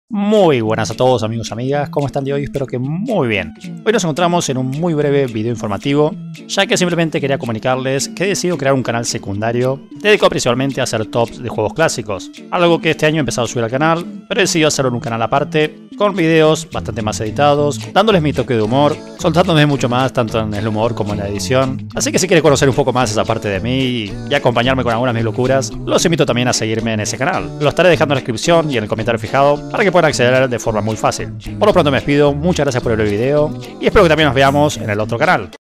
Nuevo CANAL SECUNDARIO - Video Informativo
Este es un breve video donde queria compartirles que he decidido crear un canal secundario. Estara centrado en videos de tops de juegos clasicos, dandole mi toque de humor y una edicion mas trabajada para intentar que se diviertan y pasen muy buen rato.